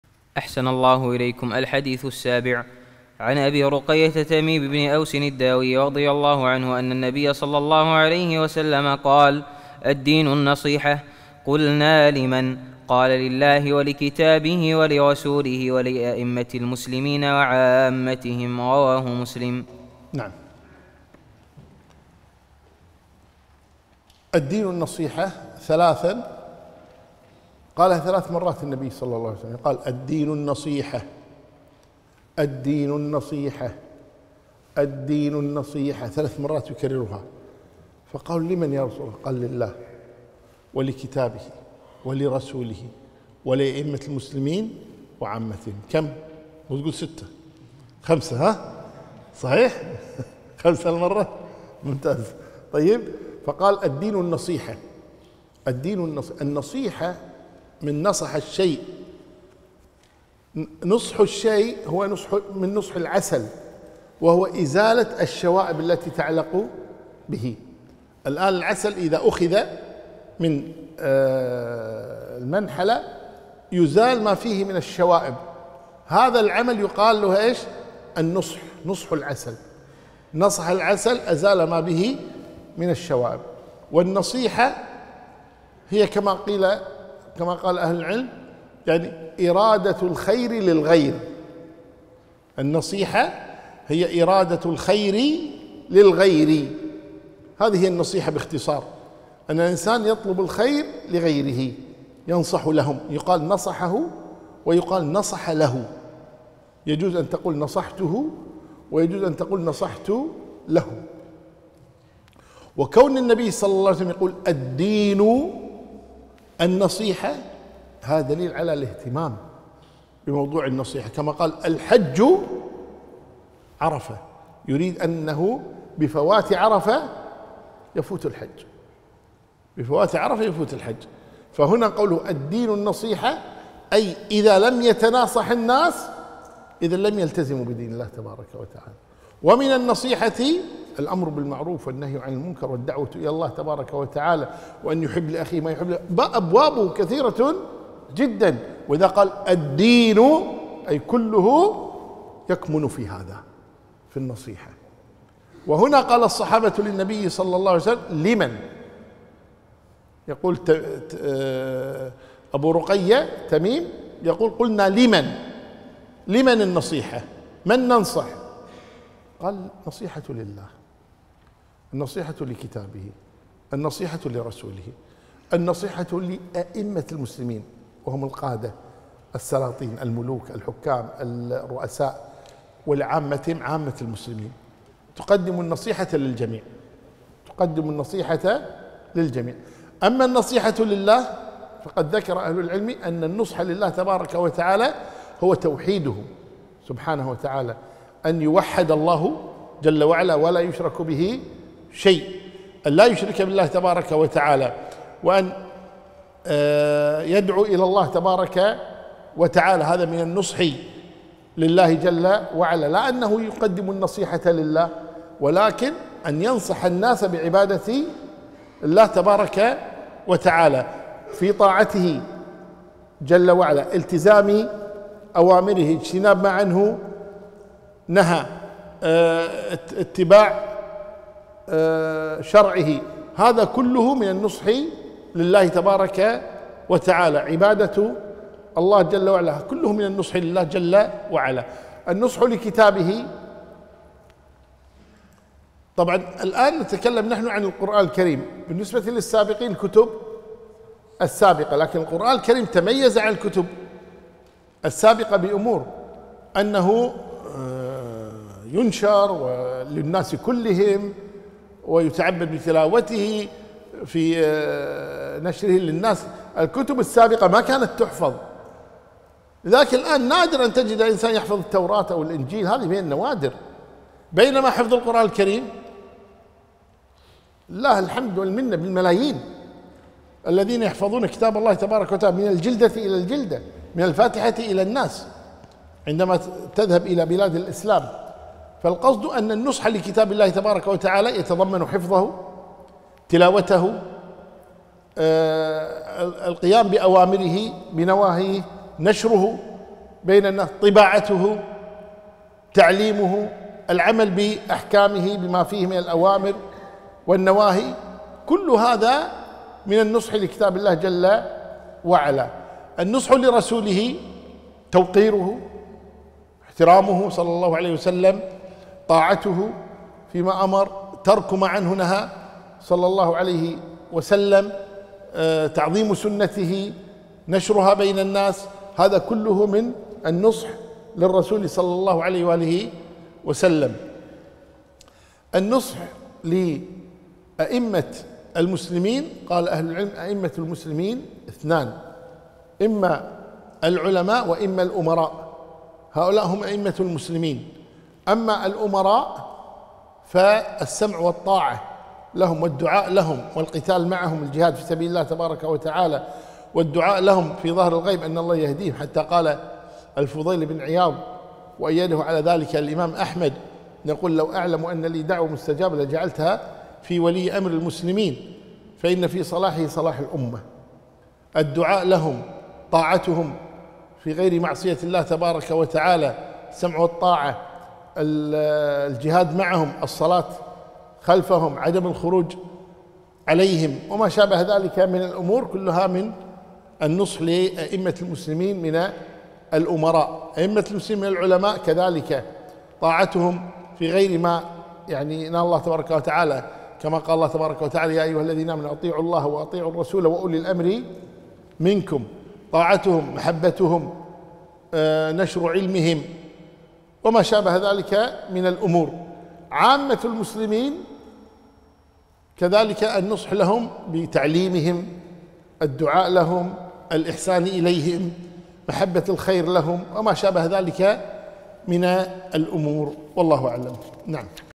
7 من 42 - الدين النصيحة I الشيخ د. عثمان الخميس I الأربعين النووية
عَنْ أَبِيْ رُقَيَّةَ تَمِيْم بْنِ أَوْسٍ الدَّارِيِّ رضي الله عنه أَنَّ النبي ﷺ قَالَ:( الدِّيْنُ النَّصِيْحَةُ قُلْنَا: لِمَنْ يَا رَسُولَ اللهِ ؟ قَالَ: للهِ، ولكتابه، ولِرَسُوْلِهِ، وَلأَئِمَّةِ المُسْلِمِيْنَ، وَعَامَّتِهِمْ (رواه مسلم.

 الكتاب الخامس
5 من 10  من برنامج مفاتح الطلب

1444 هـ - 2023 م

للشيخ الدكتور عثمان الخميس

 📕الكتاب الخامس:
الأربعين النووية في مباني الإسلام وقواعد الأحكام
تصنيف العلامة
أبو زكريا يحيى بن شرف النووي الشافعي

للتفصيل انظر الوصف باليوتيوب